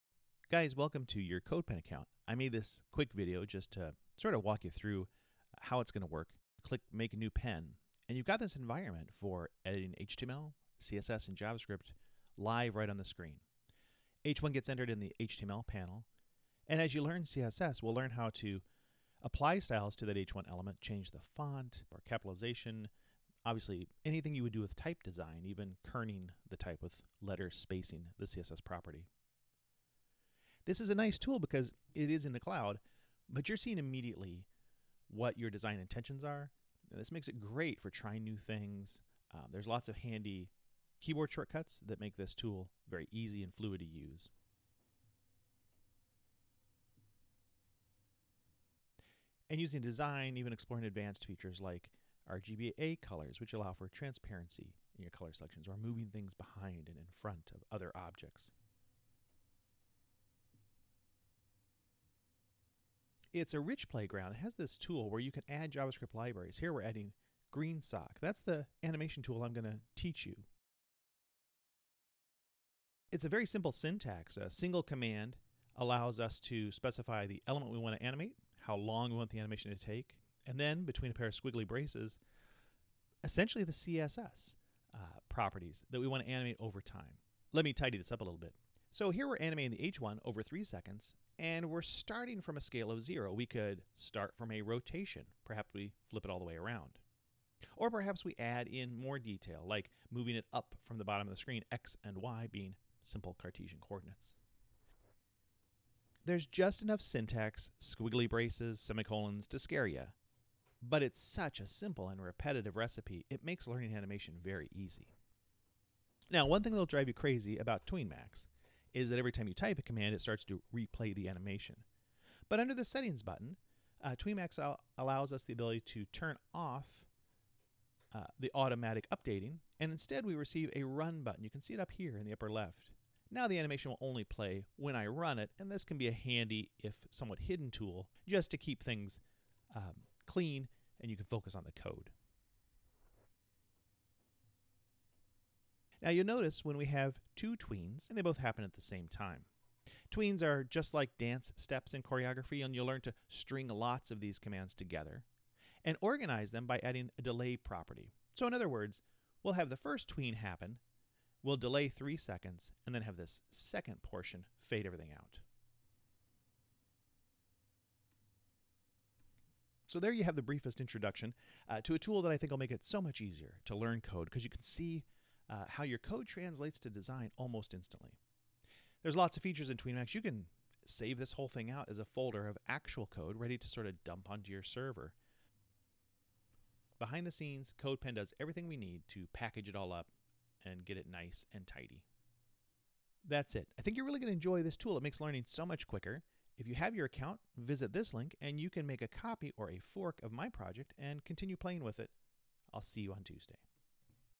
Using Codepen
Quick walkthrough of the main tool we will use to learn web design in class